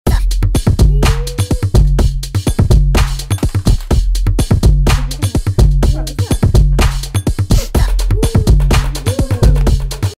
Капитан Америка Щит Капитана Америки
Это новый фокус с щитом Капитана Америки.
Супергерой Даня.